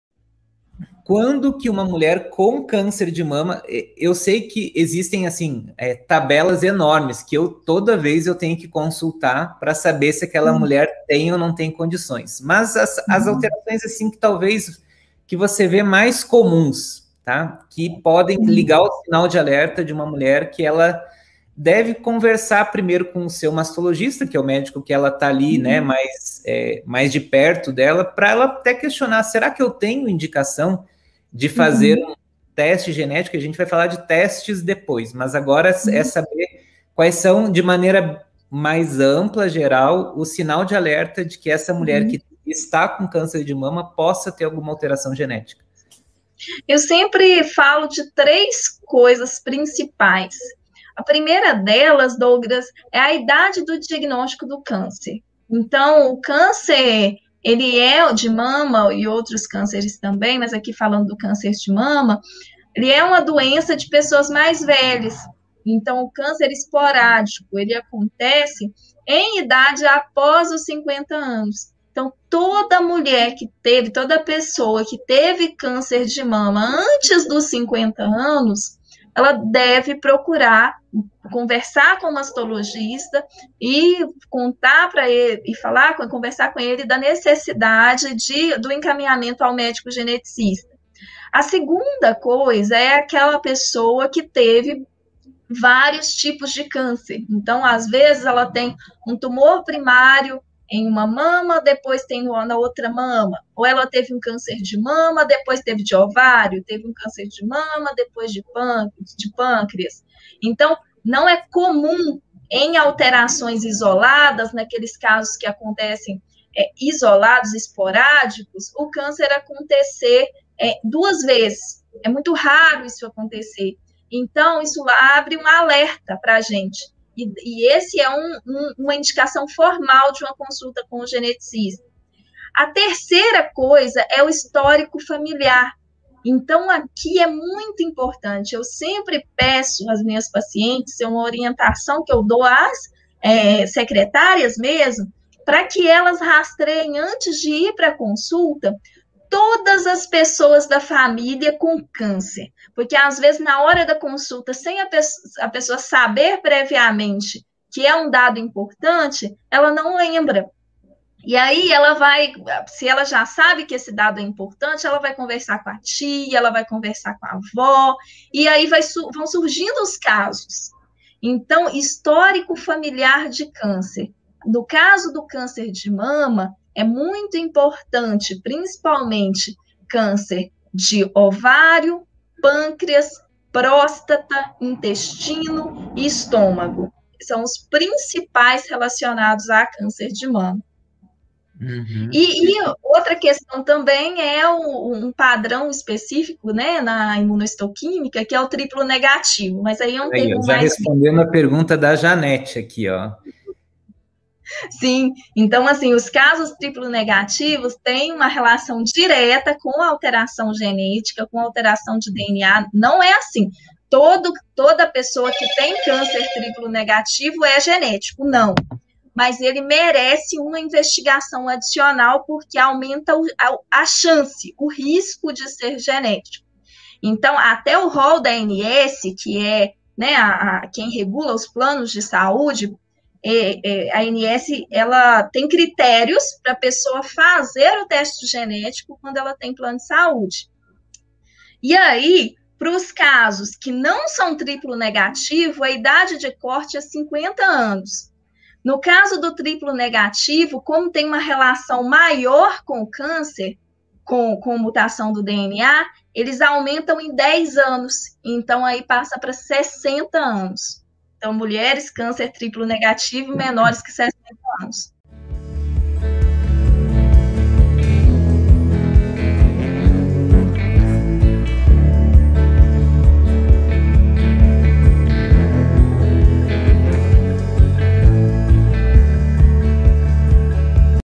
Tenho câncer de mama. Preciso fazer teste genético?
✔Dr. Douglas Soltau Gomes
CRM-PR: 26.665  - RQE: 1.635
Cascavel - PR